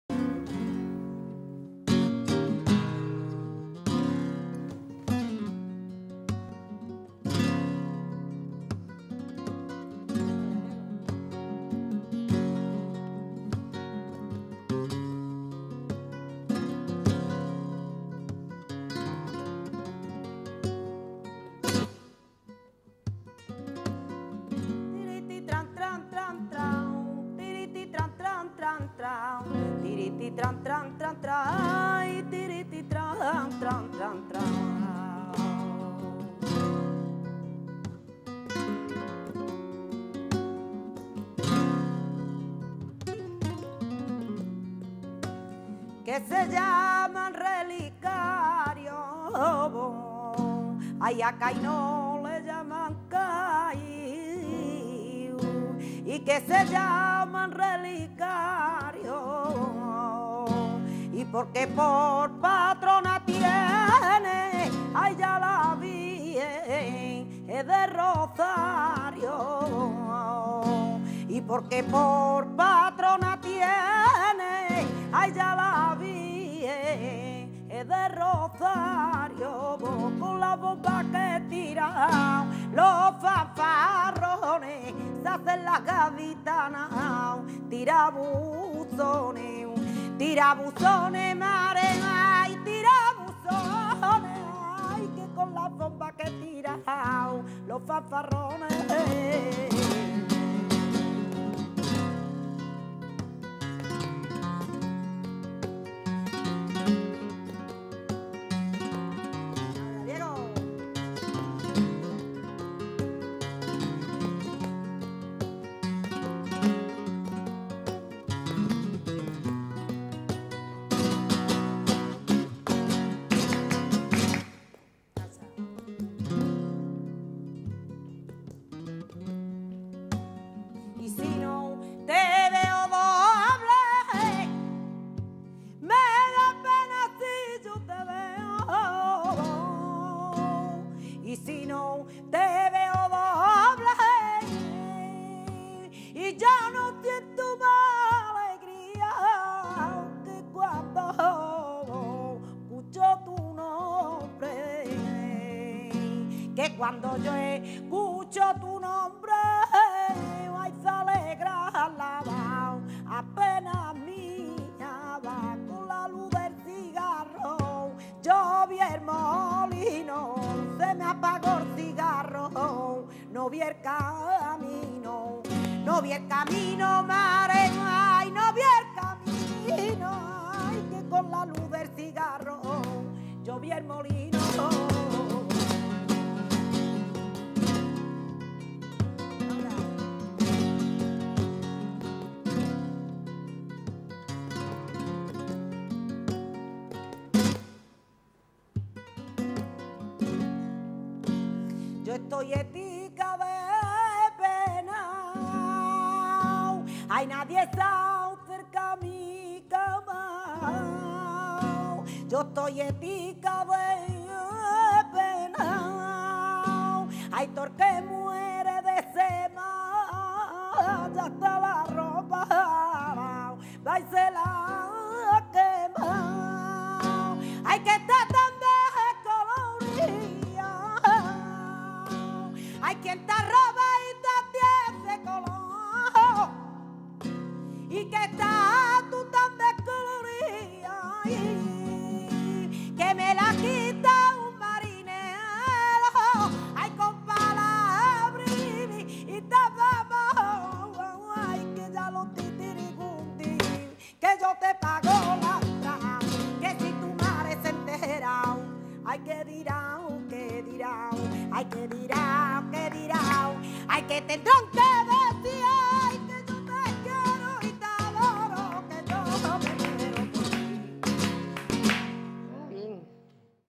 VIDEO DE AURORA VARGAS POR ALEGRIAS DE CADIZ
TEXTO: FEDERICO ALONSO PERNÍA. AURORA VARGAS NOS CANTA POR ALEGRÍAS DE CÁDIZ, CON LA GUITARRA DE DIEGO AMAYA